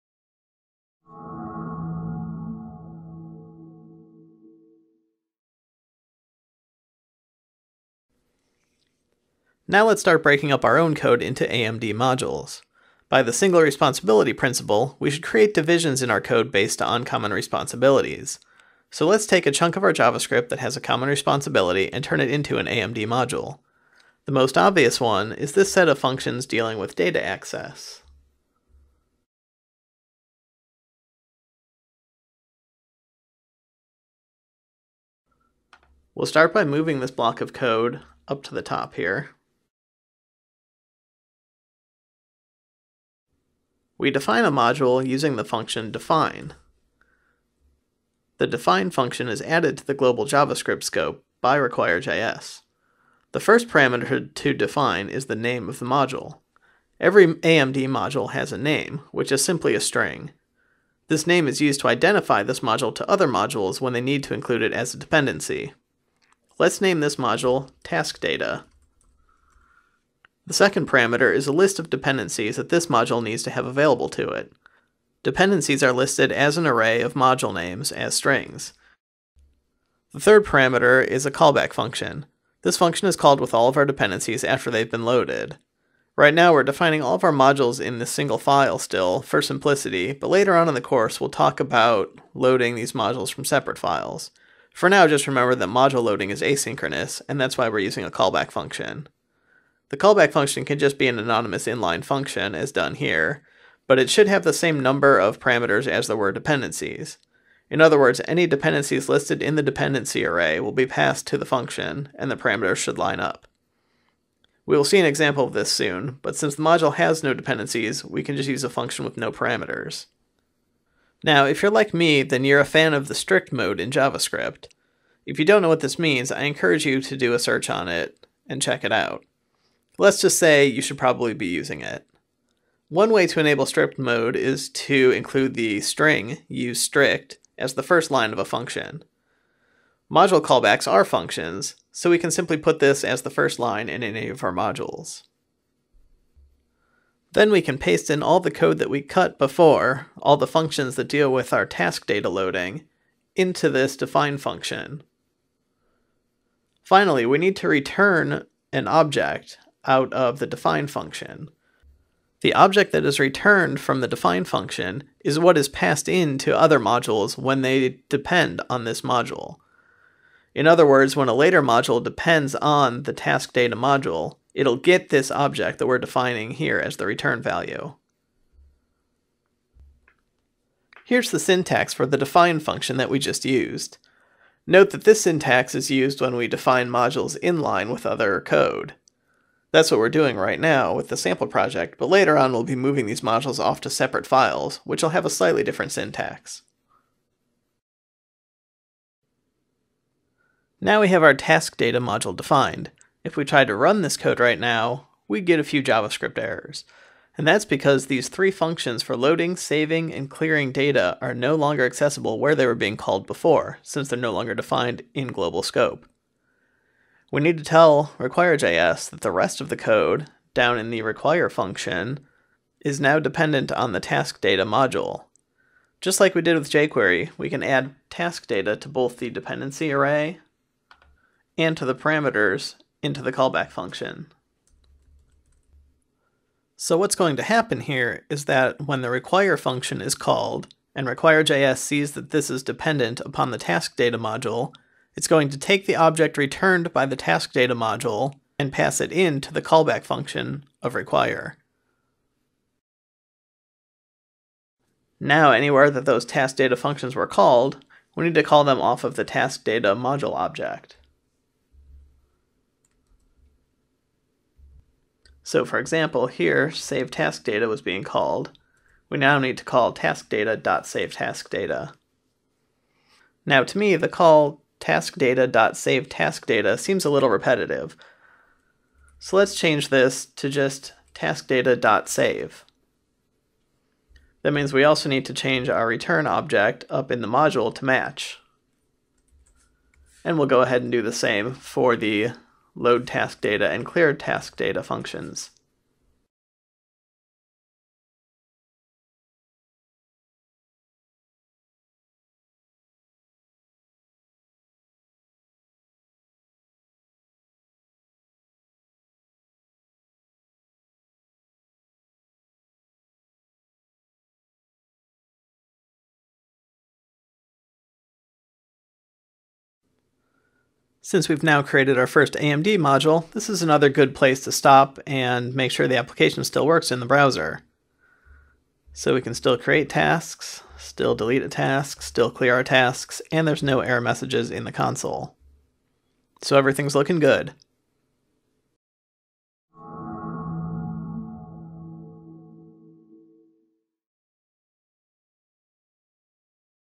Debugging and source maps in RequireJS | Pluralsight
In this video, Pluralsight author Jeff Valore introduces you to debugging and source maps in RequireJS.

The full course introduces RequireJS, a module loader and dependency injection framework for JavaScript. You'll learn how to use RequireJS to better organize your code into separate modules. You'll then learn how to use the r.js tool to optimize your code before releasing to production.